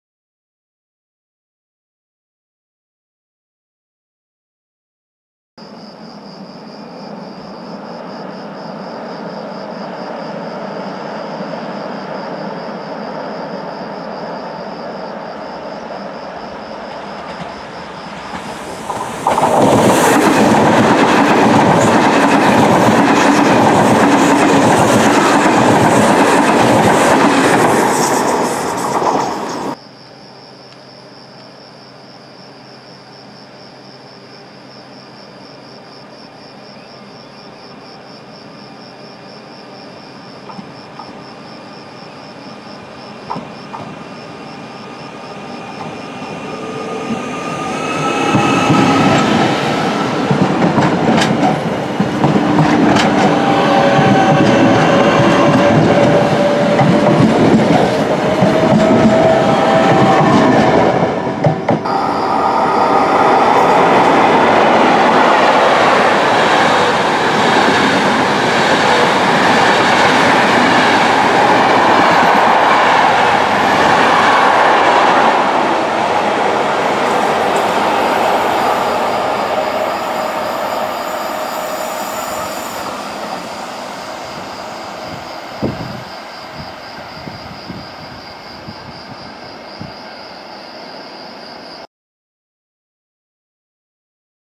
Transiti nella stazione di Latisana - 04/06/2015
In mezz'ora, al tramonto, alcuni treni in transito a Latisana:
ES 9449 Frecciargento (Roma Termini - Trieste Centrale)
Regionale 2688 (Trieste Centrale - Portogruaro-Caorle)
ES 9741 Frecciabianca (Milano Centrale - Trieste Centrale)